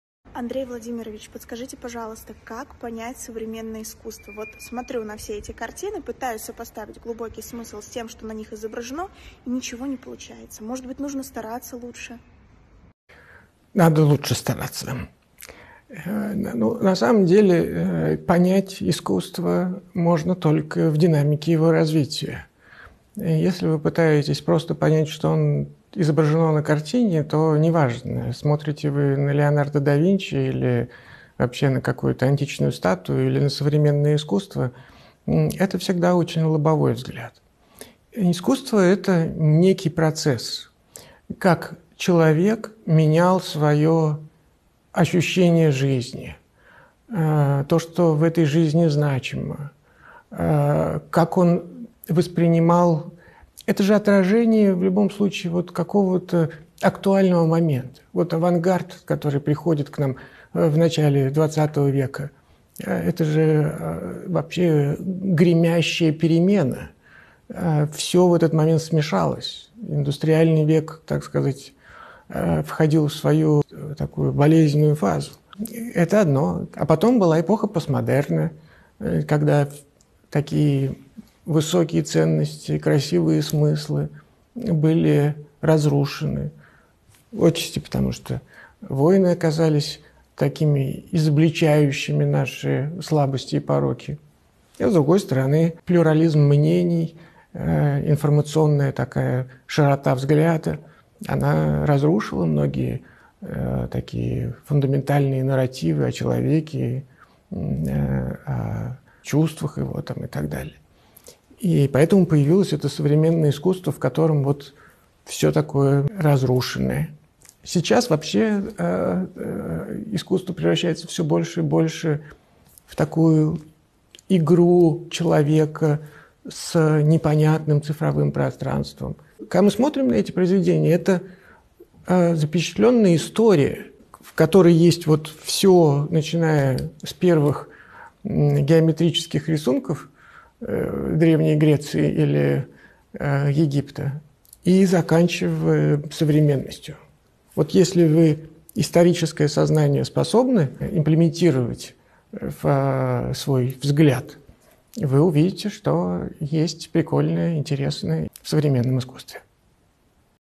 Как понять современное искусство? | Андрей Курпатов
kurpatov_official: «Никто не спорит с тем, что современных авторов можно копировать. Никто не спорит и с тем, что современное искусство — это “сложно и непонятно”. И никто не спорит с тем, что здесь вопрос совершенно в другом.

В галереях не спрашивают: понимаете ли вы то, что изображено; там также не спрашивают: смогли бы вы повторить произведение. Ведь понять искусство можно только в динамике его развития. Если вы пытаетесь понять что изображено на картине, вам следует обратиться в прошлое, в первоисточник зарождения образов. Так как искусство — это запечатленная история, в которой есть все: от совершенно простых форм и сочетаний до совершенно сложных абстракций и композиций”.

Произведение творца — завораживающий процесс, берущий своё начало от мироощущения, значимости жизни, роли любви и вопроса человечества. Иными словами, его произведение — это отражение актуальности мирских вопросов.

Задайте себе вопрос: что делает эту работу достойной того, чтобы она оказалась в картинной галерее? Если вы не можете ответить на этот вопрос, значит вы чего-то не знаете! А осознание своего незнания — это самое важное, именно оно открывает перед нами перспективы для совершенствования наших навыков и умений».

Цитата из книги «Красная таблетка. Посмотри правде в глаза!»